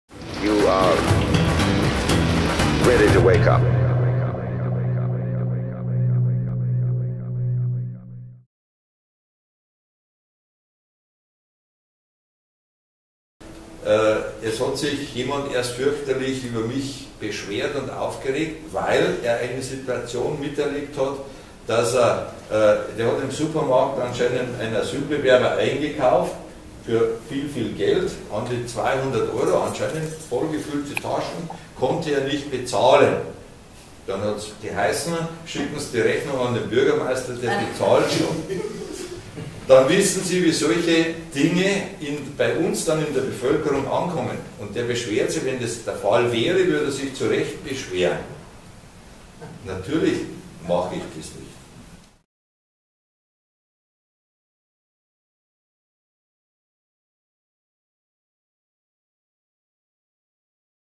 Geisenfeld Asyl -Asylbewerber zahlt nicht - Zahlt nun der Bürgermeister?
Wenn ein Asylbewerber in Geisenfeld an der Supermarktkasse nicht zahlen kann, zahlt dann der Bürgermeister für ihn? Natürlich nicht, stellt der Bürgermeister klar. ( Leider ein nicht aus der Welt zu schaffender "Gehirnschoaß", wie man in Bayern zu Gerüchten sagt)